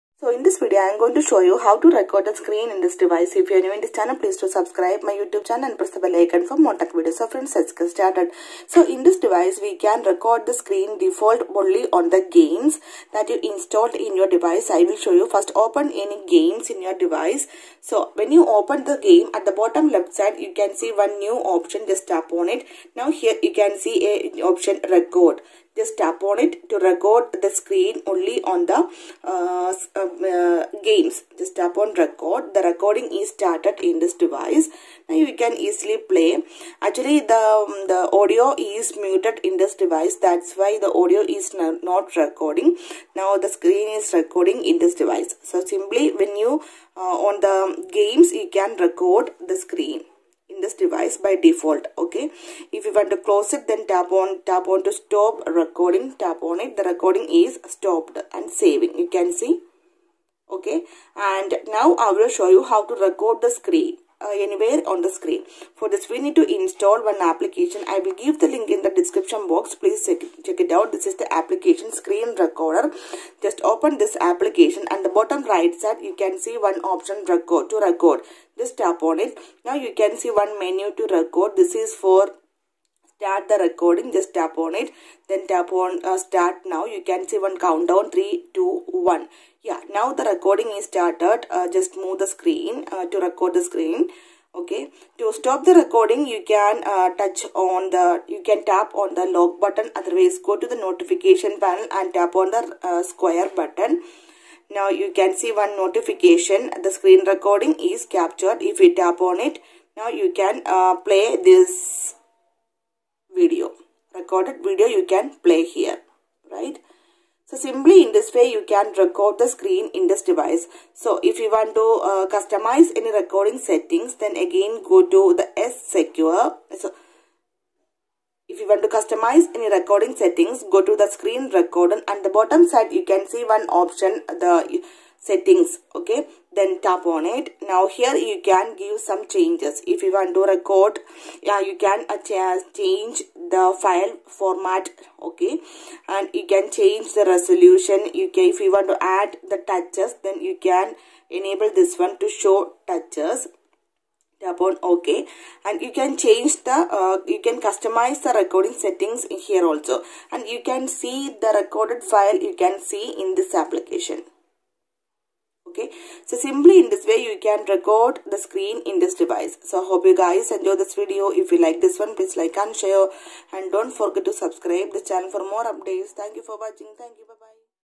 Samsung Galaxy M42 5G | Screen recording Full guide in Samsung Galaxy M42 5G
Hi friends this video shows you that " How to do screen recording in Samsung Galaxy M42 5G "